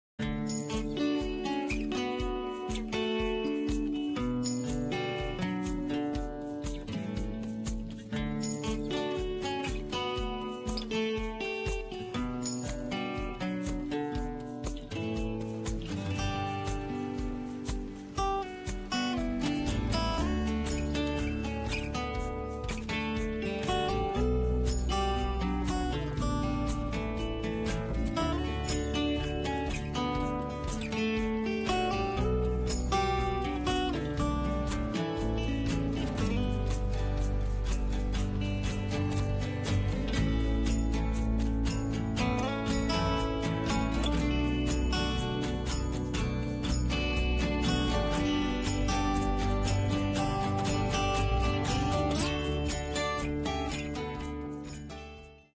Cruise Into Marketing
Take Your Marketing Training while on a Cruise. Who says business can't fun. Learn and then Relax.